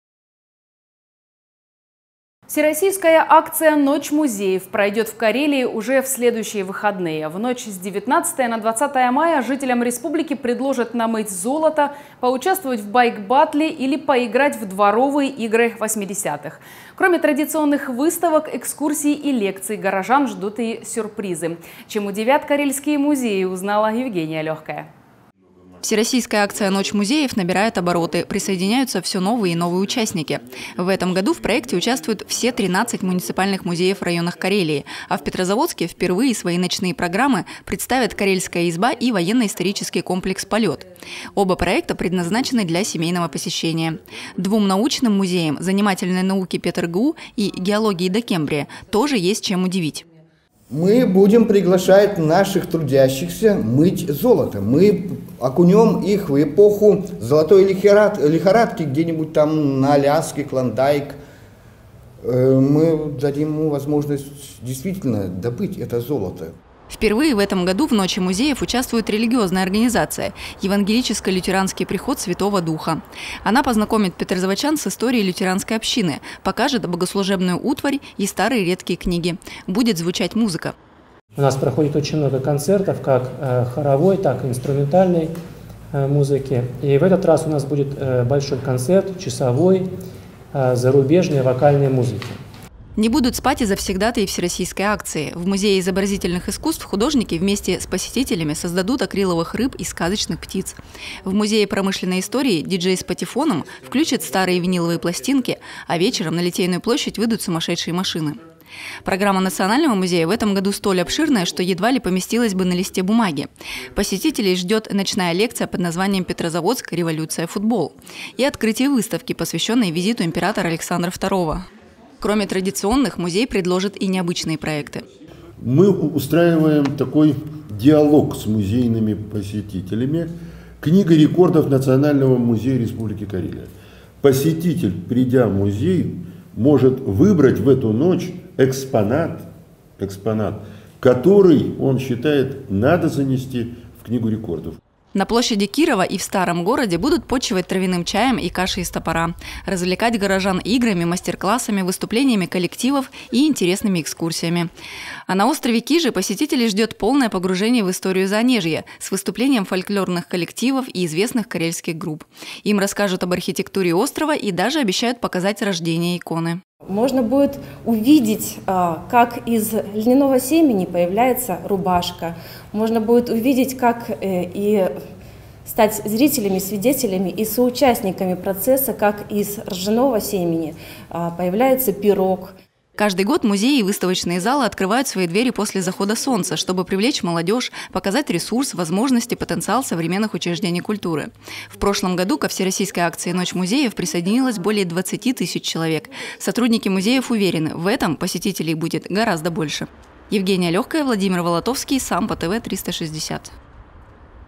Ночь музеев
Всероссийская акция «Ночь музеев» пройдет в Карелии уже в следующие выходные. В ночь с 19 на 20 мая жителям республики предложат намыть зОлота, поучаствовать в байк-баттле или поиграть в дворовые игры 80-х. Кроме традиционных выставок, экскурсий и лекций - горожан ждут и сюрпризы. Чем удивят карельские музеи, узнала Евгения Лёгкая.